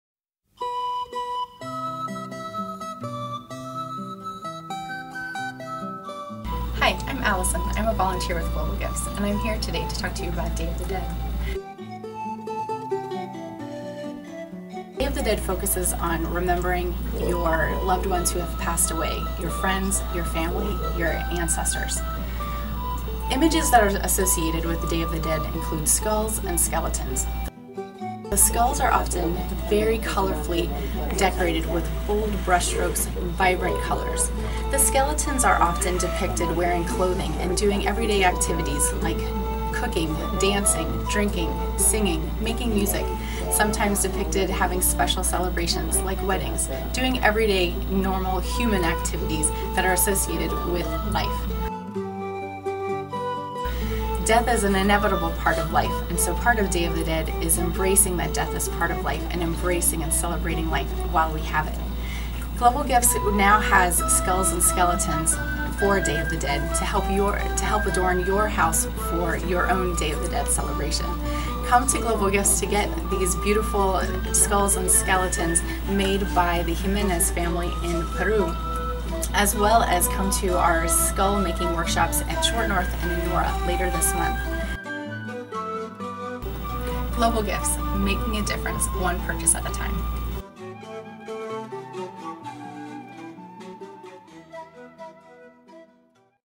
Shop Day of the Dead Items at Global Gifts
In honor of Day of the Dead (Dia de los Muertos), Global Gifts will be selling fair trade skeleton figurines handcrafted by the Jimenez family in Ayacucho, Peru. In this video, volunteer Alison talks about the origins and significance of Day of the Dead, which is celebrated in Mexico and many other parts of the world, as well as features some of the beautifully crafted skeleton figurines.